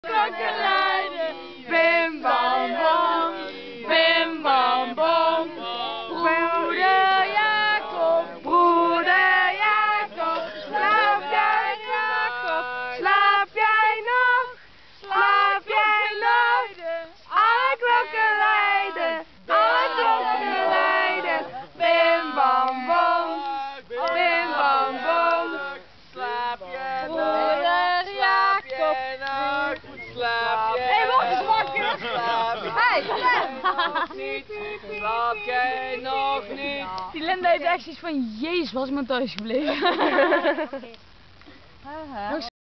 Kampvuur
aangeschoten liedjes zingen